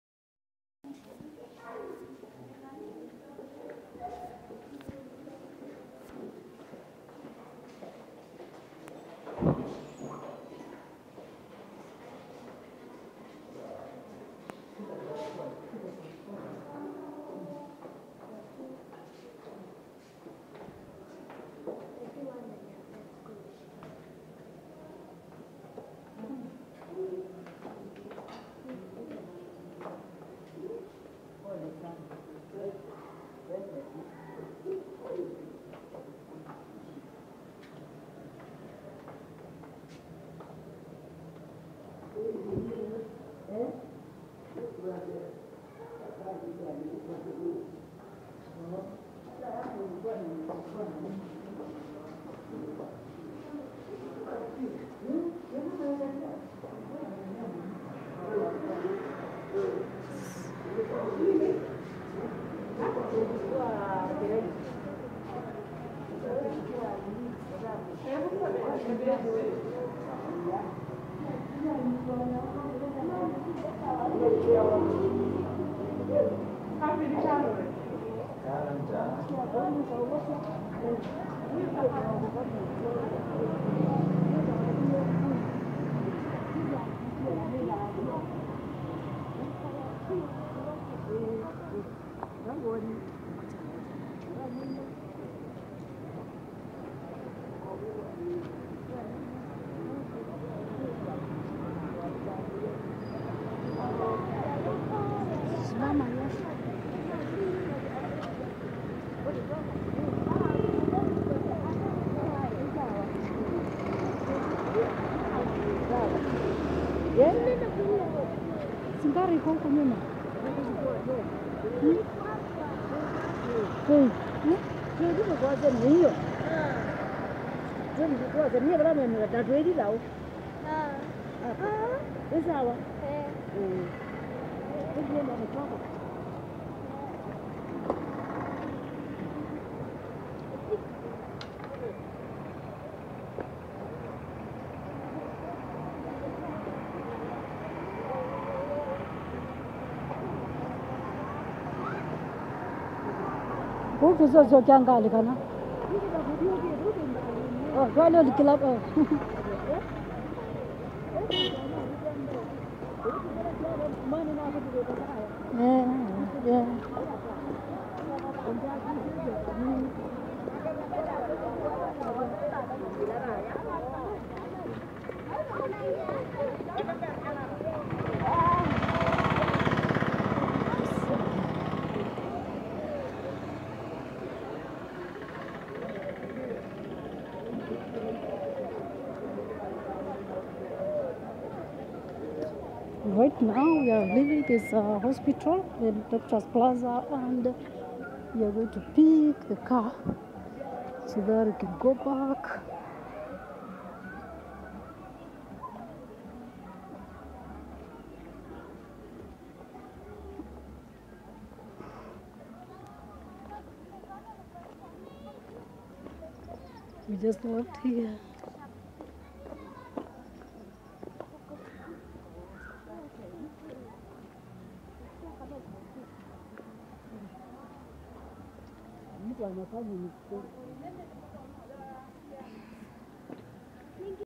A visit to the most visited hospital Machakos Doctors plaza
A tour of machakos county, MercyNguku The Kenyan Traveller ✈️🇰🇪🐘🚁 With ✈️🇰🇪